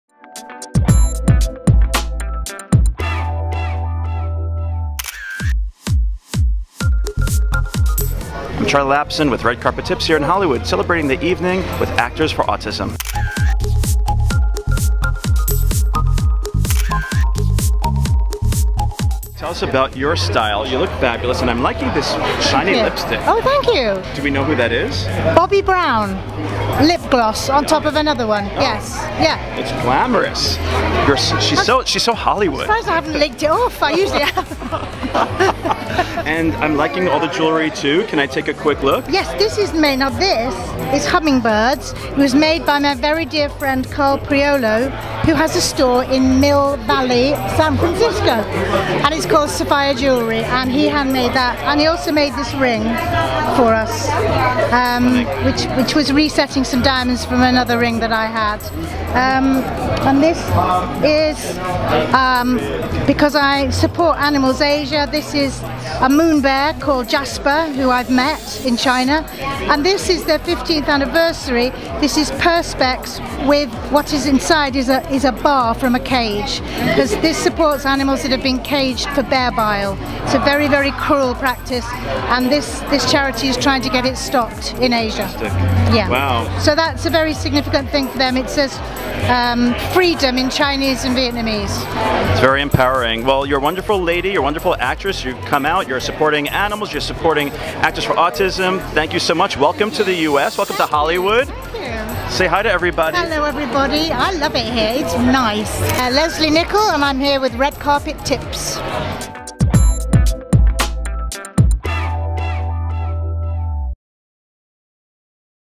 Lesley Nicol from "Downton Abbey" Breaks Down Her Style
Looking very lovely, Lesley Nicol showed off her jewelry--which supports a great cause.